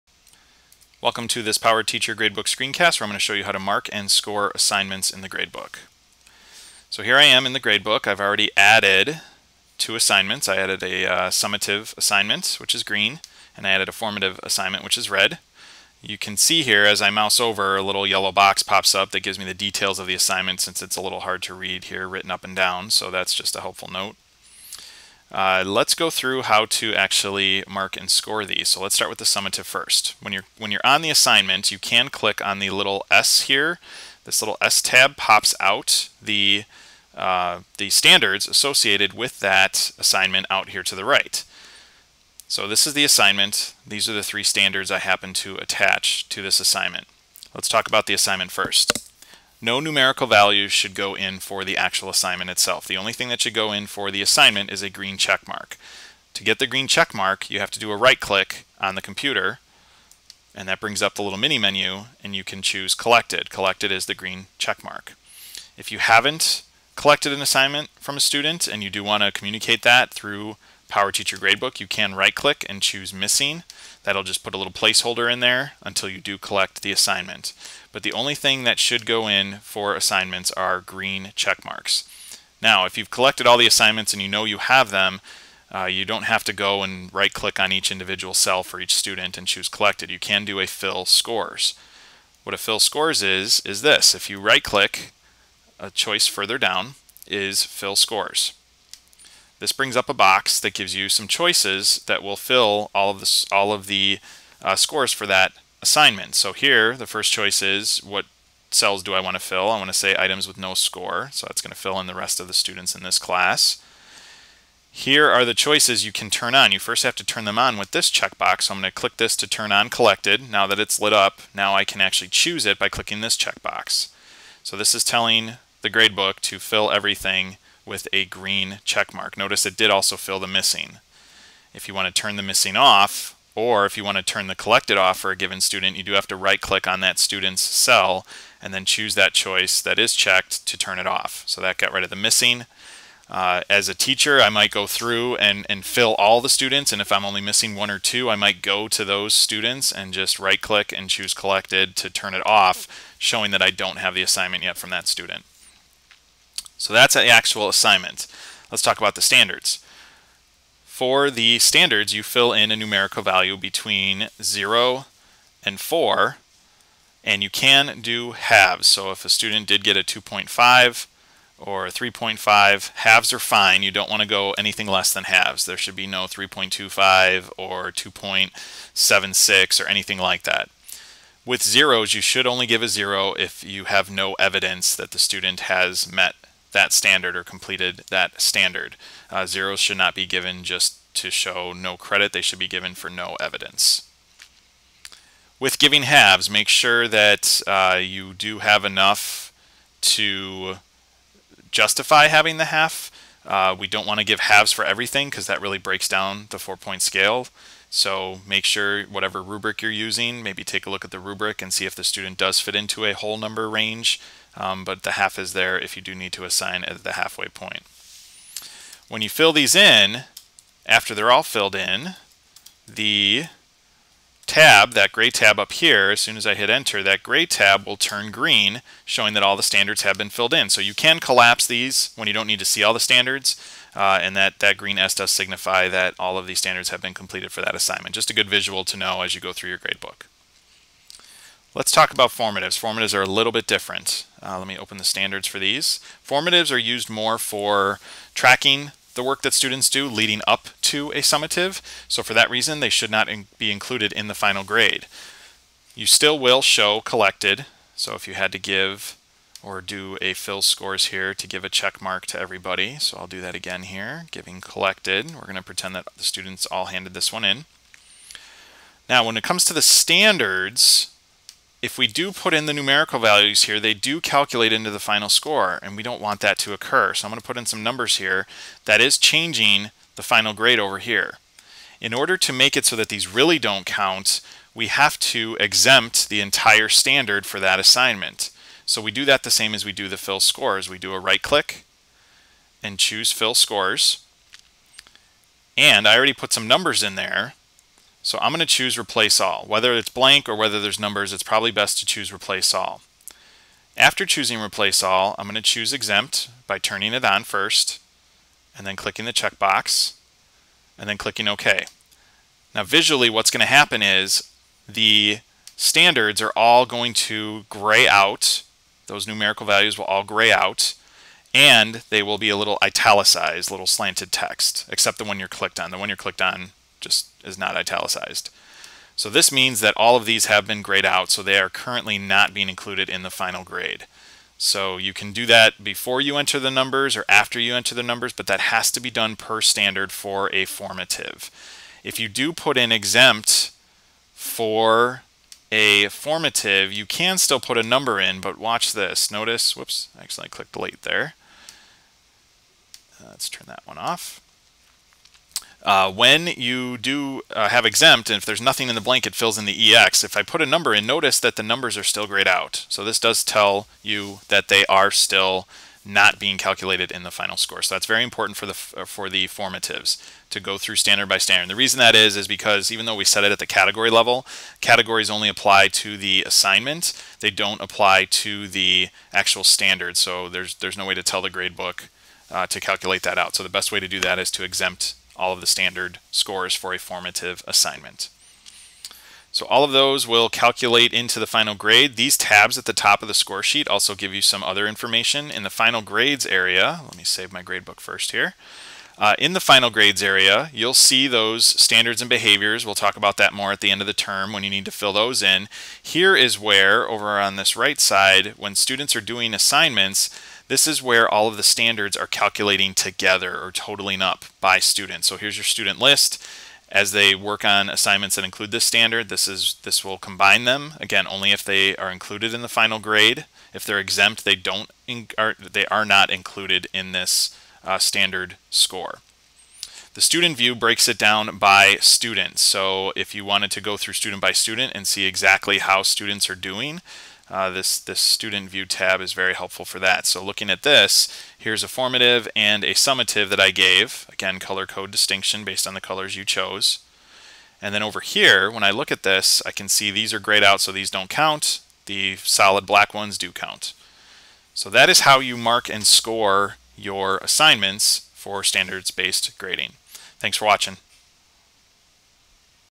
PTG - Marking & Scoring Assignments
Shows teachers how to mark and score assignments for Standards Based Grading in PowerTeacher Gradebook.